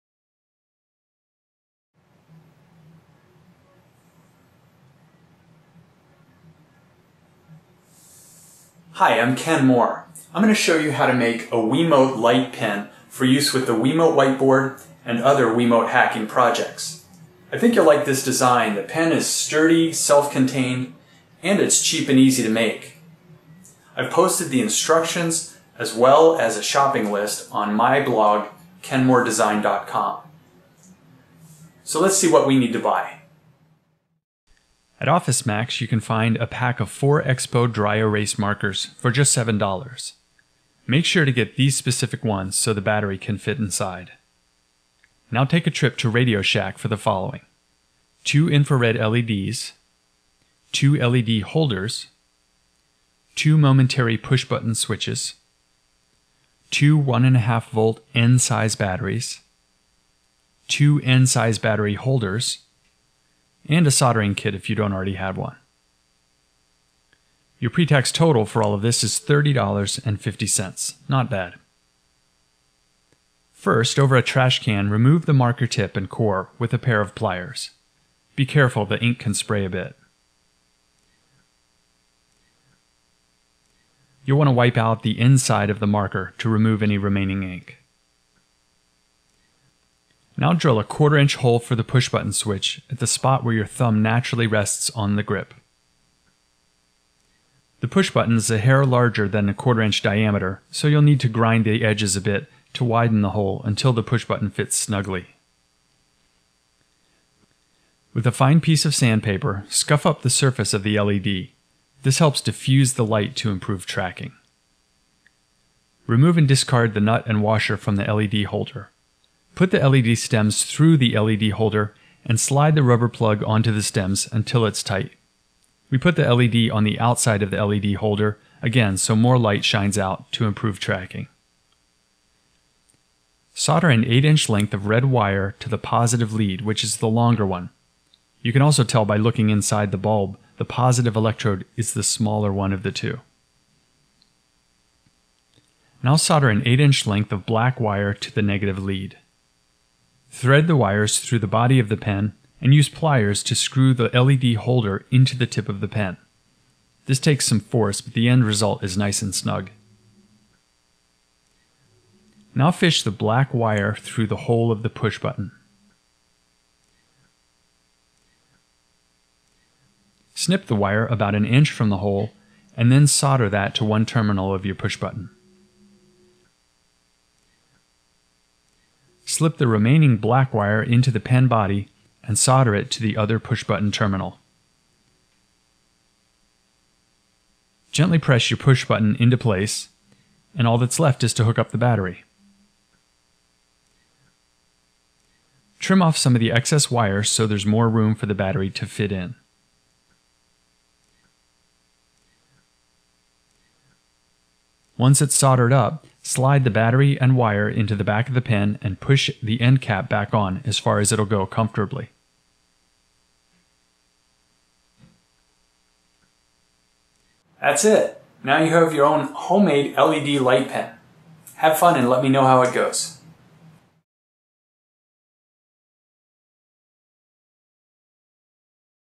Making an infrared LED light pen (for Wiimote Whiteboard)
If you want to experiment with the Wiimote Whiteboard or other wii hacking projects, you should start by making your own infrared light pens.  This design is sturdy, self-contained, and cheap and easy to make.  Video shows step-by-step instructions plus shopping list.